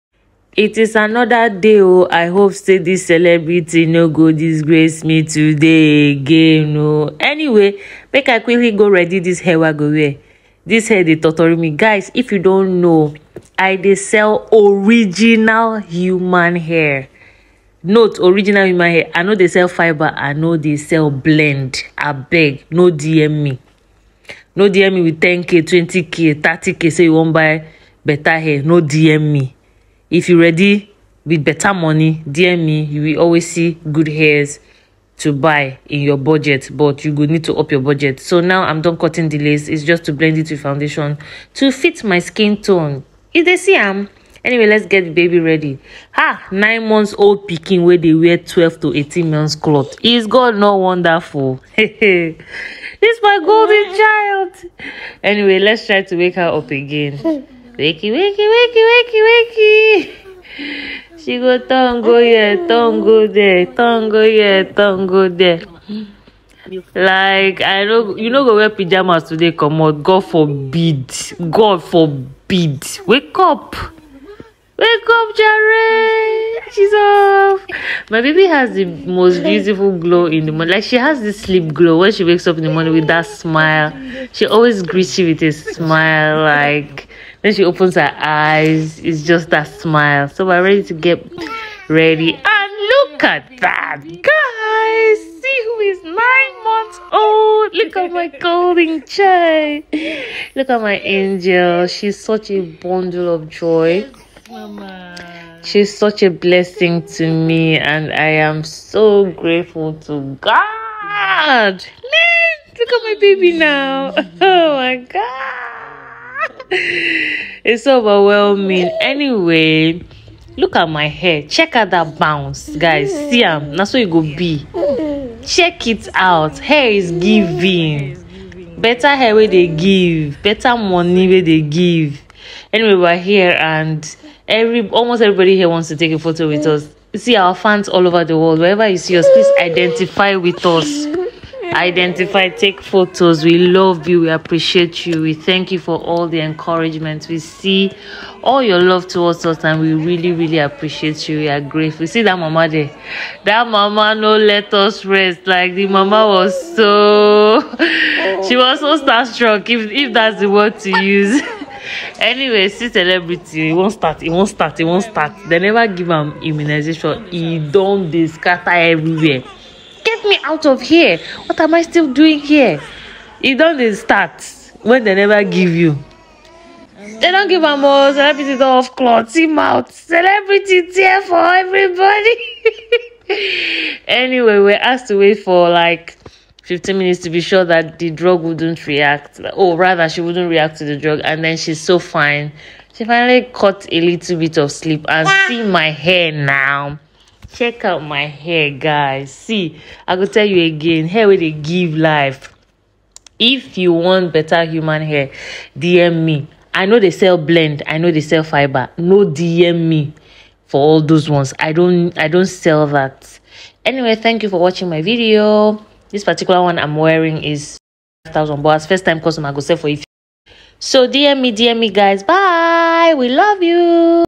Shades of Diadem, The 9 months version.
The baby is now an infant giving me different shades everyday. Postpartum struggles are over, we now enjoy motherhood 🥰😊😍